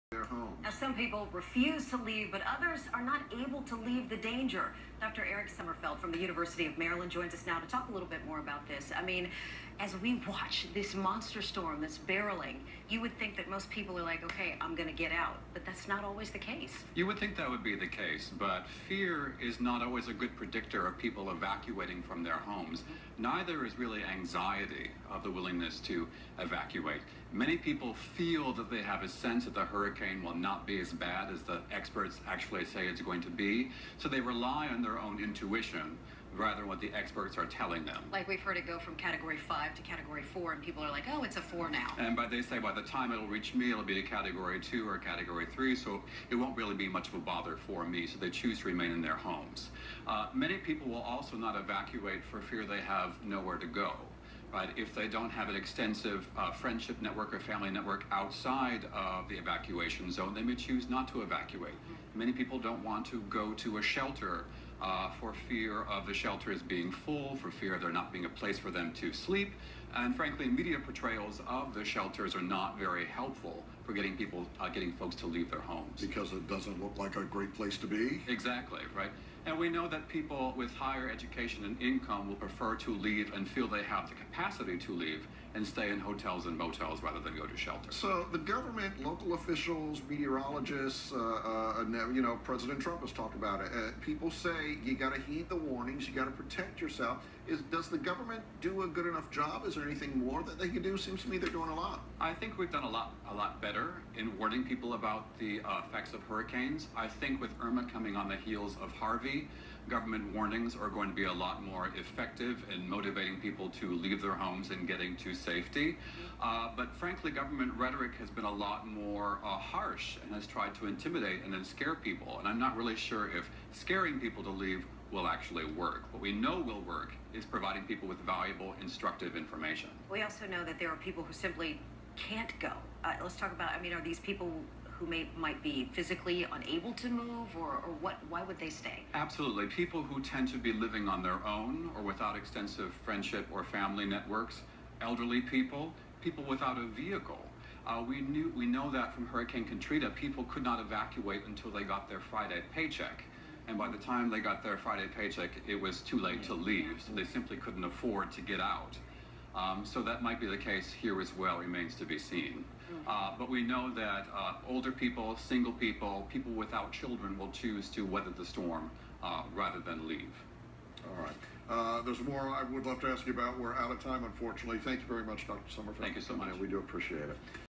Dr. Sommerfeldt on Fox5
Erich Sommerfeldt, Ph.D. talking about why people don't evacuate during disasters i.e. hurricane Irma.